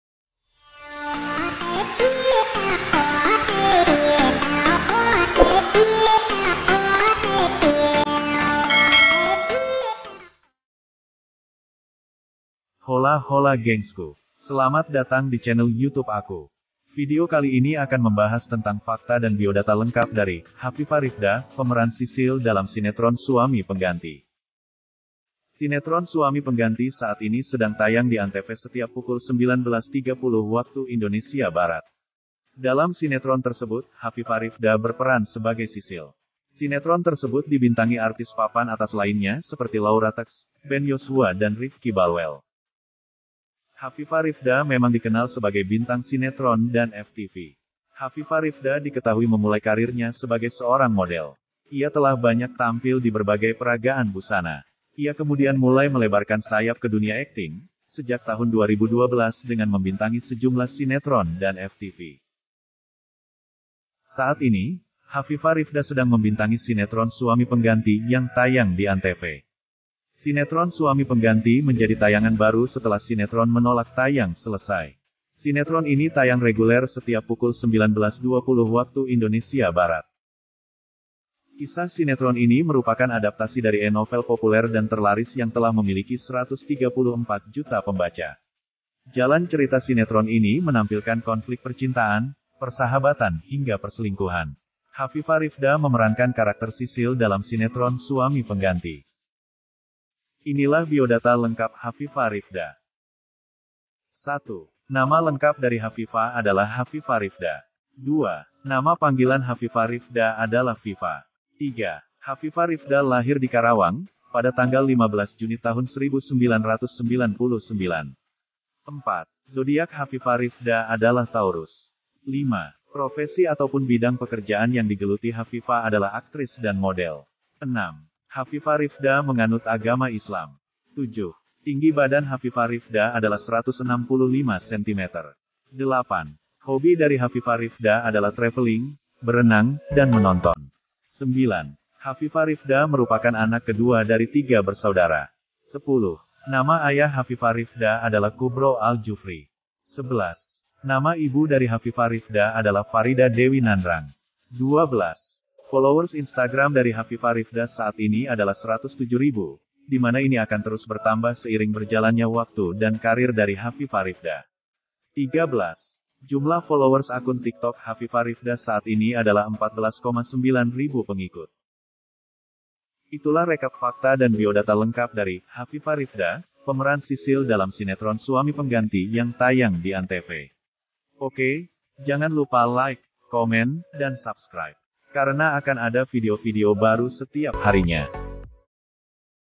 PROFIL & BIODATA HAVIVA RIFDA‼️LENGKAP UMUR & AGAMA || PEMERAN SISIL DI SINETRON SUAMI PENGGANTI⁉️
Holla Holla Gengsku!!
Selamat datang di Channel Youtube aku.
Vidio kali ini akan membahas tentang Fakta dan Biodata lengkap dari "Haviva Rifda" pemeran Sisil dalam sinetron Suami Pengganti.

Mohon maaf yang sebesar-besarnya jika ada informasi yang kurang tepat, dan jika ada kesalahan kalian dapat mengoreksinya dikolom komentar ya.
Terimakasih 😍

Selamat menonton!! Semoga menghibur dan bermanfaat yaa!!

Komen di bawah Vidio apa yang akan dibuat berikutnya??

Thankyouuu💜